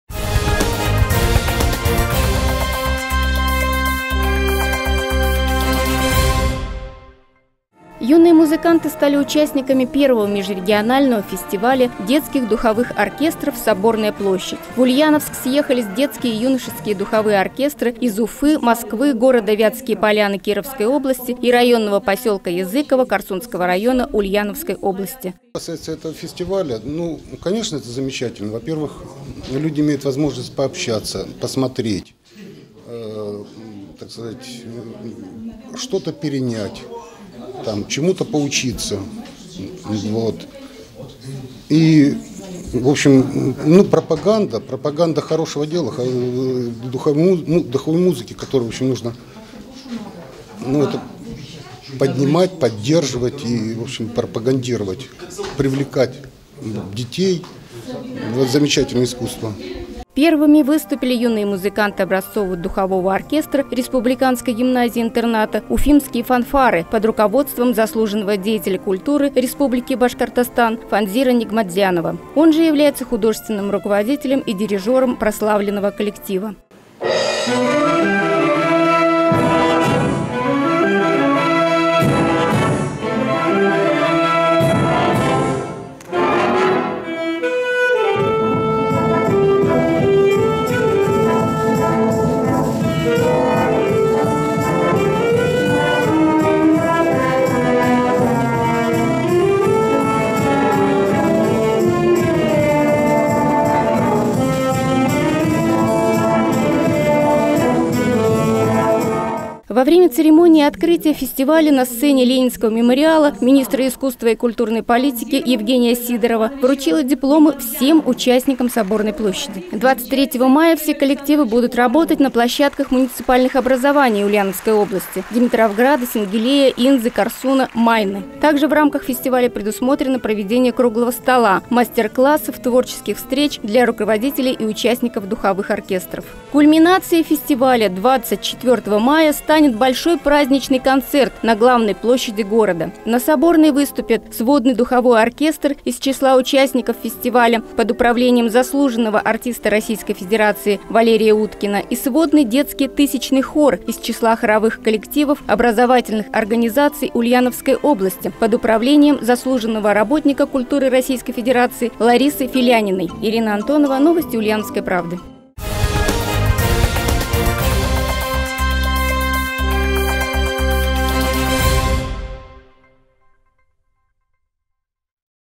Детские духовые оркестры России съехались в Ульяновск на фестиваль «Соборная площадь»
В Ульяновской области открылся Межрегиональный фестиваль детских духовых оркестров «Соборная площадь». За три дня фестиваля своё творчество представят 7 коллективов из 4 регионов страны. Фестиваль проводится по инициативе Ассоциации духовых оркестров и исполнителей на духовых и ударных инструментах «Духовое общество» имени Валерия Халилова и Правительства Ульяновской области.